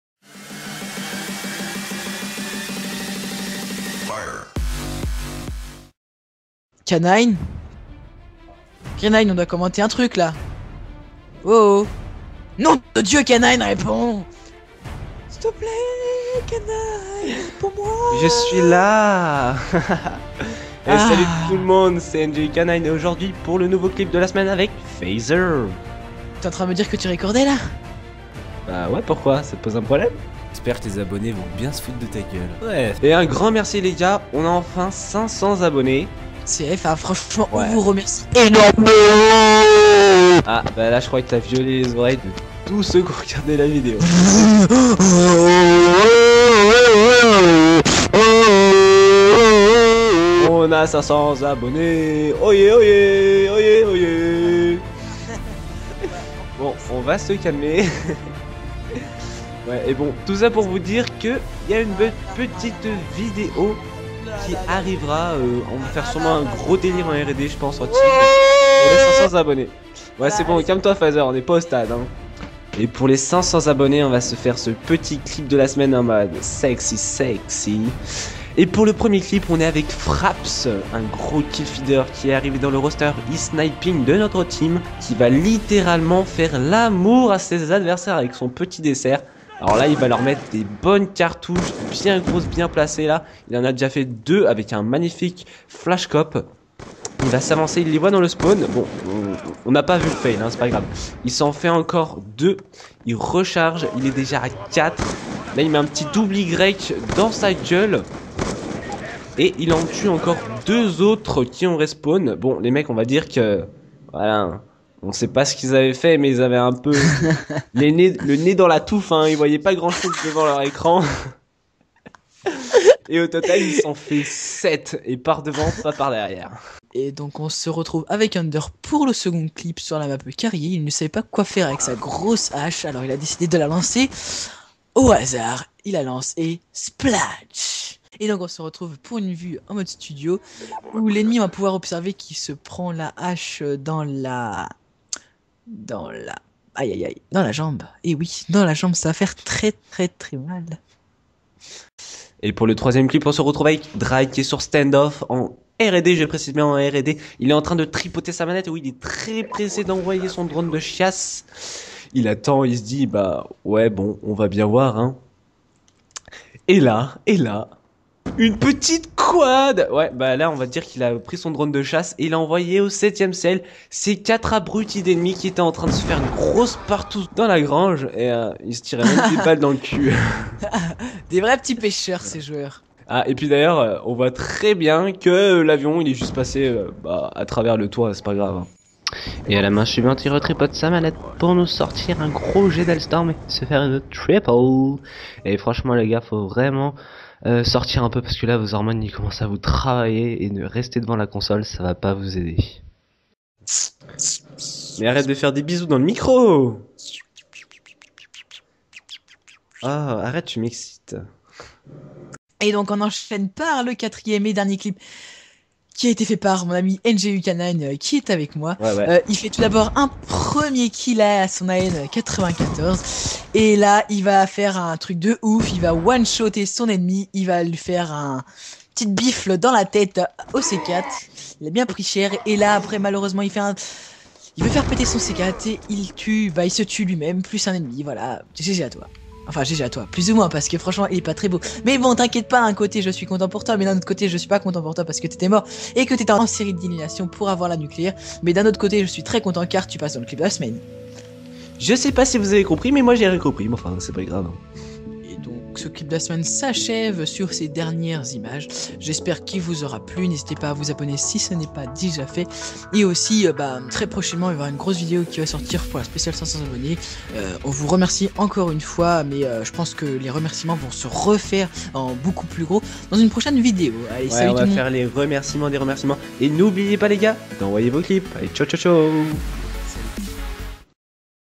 [GRAND WTF]♔Clip de la semaine #7♔
●▬▬▬▬▬▬▬▬▬▬● EverPlay ●▬▬▬▬▬▬▬▬▬▬●

Salut à tous merci pour les 500 abonnés,on se retrouve une nouvelle fois pour le clip de la semaine avec EvP nGu k9. Nous espérons qu'il va vous plaire et pensez à nous envoyer vos clips à l'adresse suivante si vous voulez passer et vous faire un peu de pub car votre chaîne sera dans la description